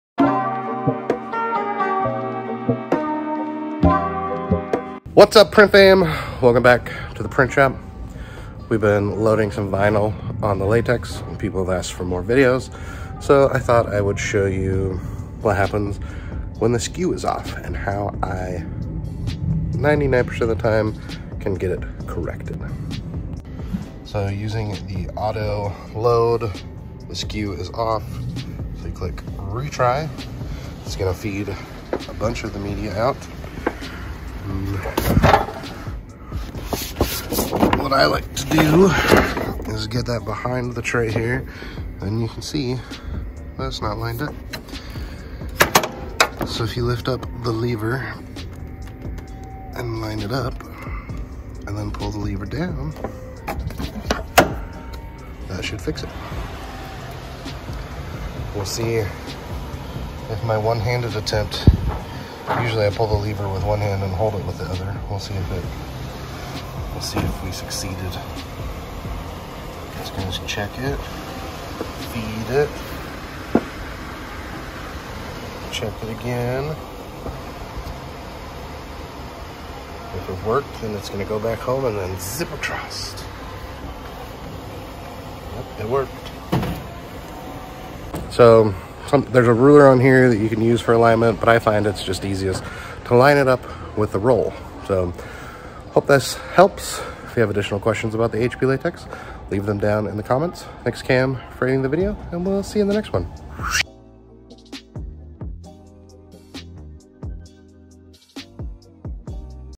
HP Latex, fix skew on media loading
Quick and simple video on how I fix the skew when loading media. Works almost every time to fix the media alignment.

My Wish List

Summa, HP, and Epson sales rep
Garrett Scotland

Buy a Melco Embroidery Machine
Todd Eggersman

Edited by Cam